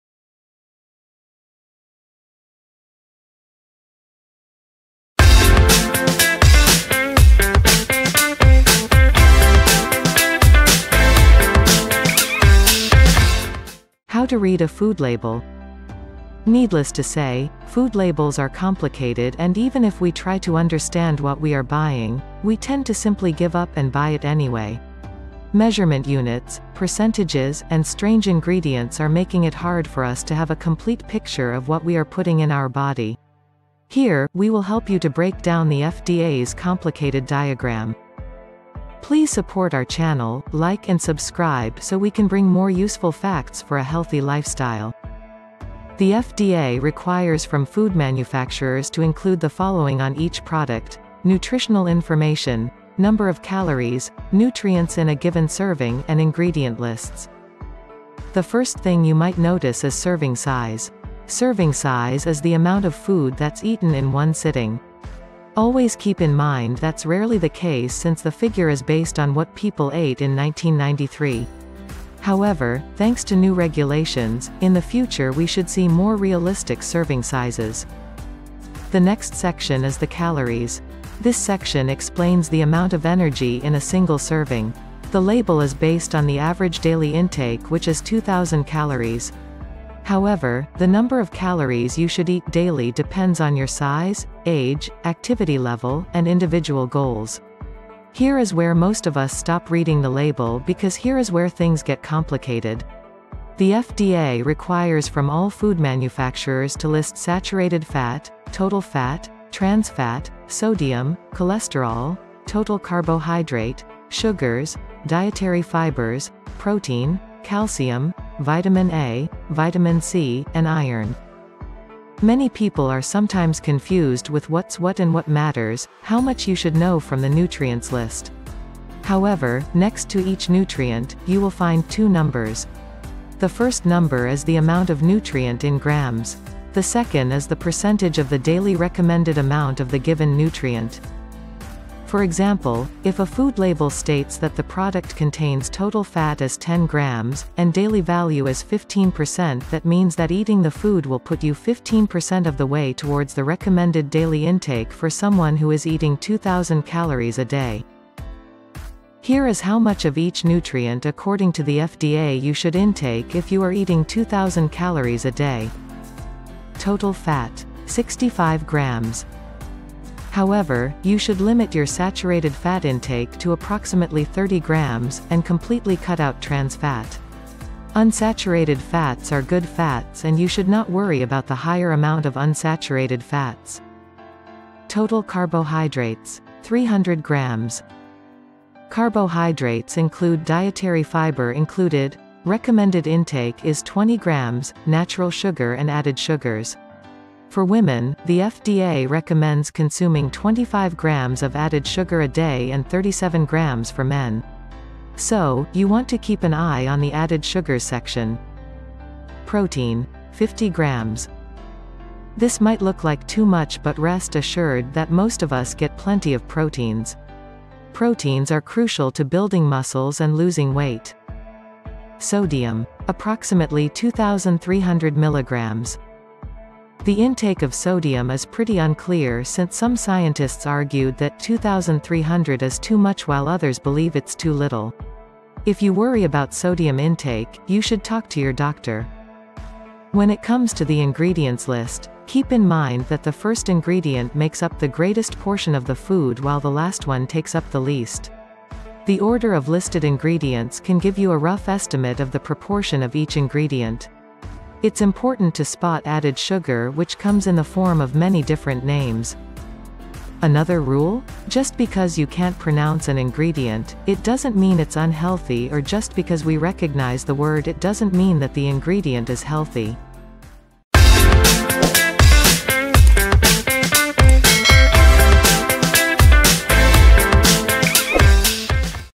How to Read a Food Label
Needless to say, food labels are complicated and even if we try to understand what are we buying, we tend to simply give up and buy it anyway. Measurement units, percentages, and strange ingredients are making it hard for us to have a complete picture of what we are putting in our body.

The FDA requires food manufacturers to include the following on each product: nutritional information, number of calories, nutrients in a given serving, and ingredient lists.

Here, we will help you to break down the FDA’s complicated diagram...